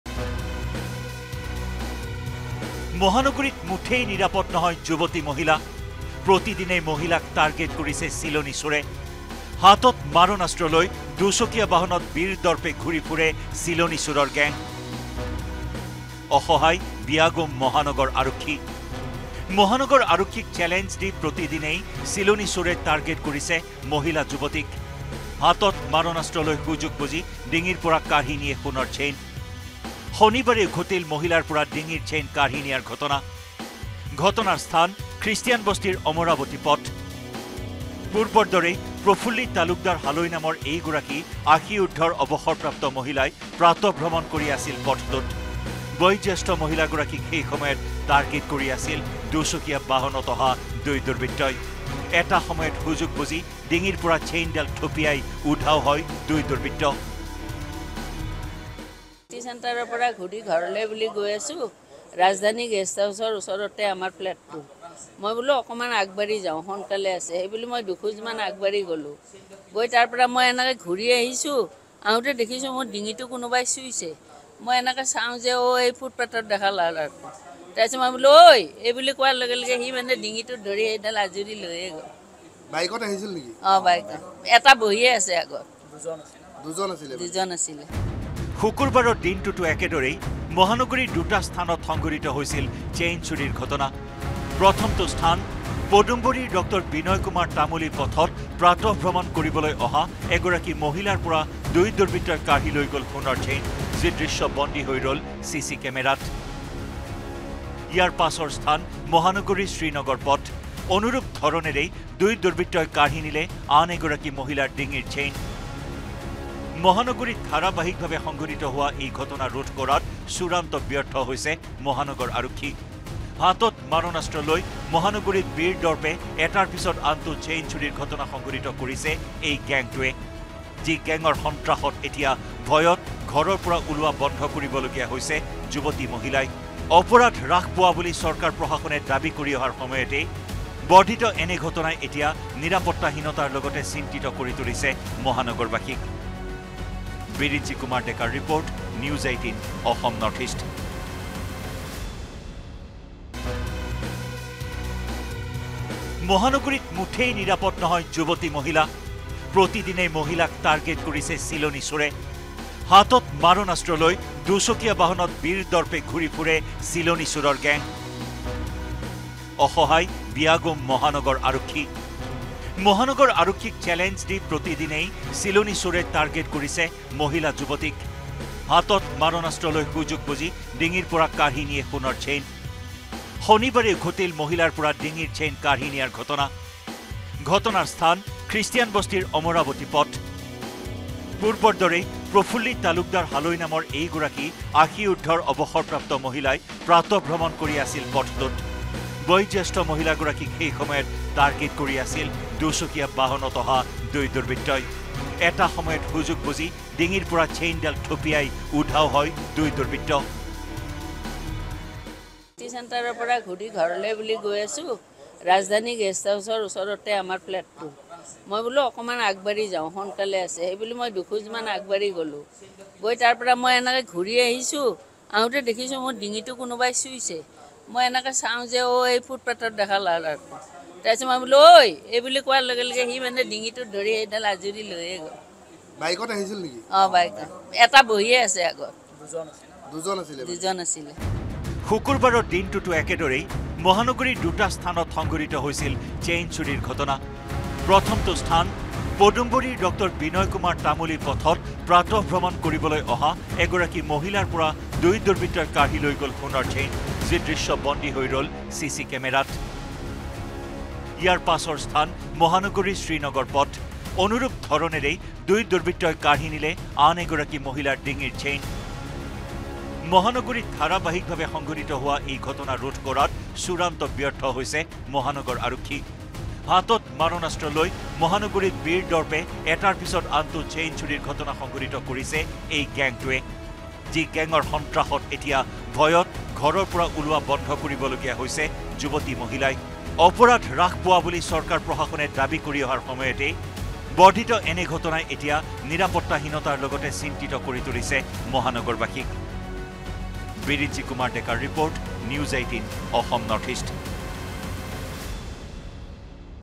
Chain Snatcher News : মহানগৰীত নিৰাপত্তাহীনতাত যুৱতী-মহিলা | N18V
Chain Snatcher News | মহানগৰীত ধাৰাবাহিকভাৱে সংঘটিত হৈছে মহিলাৰ ডিঙিৰ চেইন চুৰিৰ ঘটনা। এটা সপ্তাহত মহানগৰ আৰক্ষীক চেলেঞ্জ দি চিলনী চোৰে সংঘটিত কৰিছে পাঁচটাকৈ চুৰিৰ ঘটনা ৷ শনিবাৰে পুৱাই খ্ৰীষ্টানবস্তিৰ অমৰাৱতী পথৰ পৰা থপিয়াই নিলে এডাল সোণৰ চেইন ৷ কিন্তু কিয় আৰক্ষীৰ জালত নপৰে এই চিলনী চোৰ ৷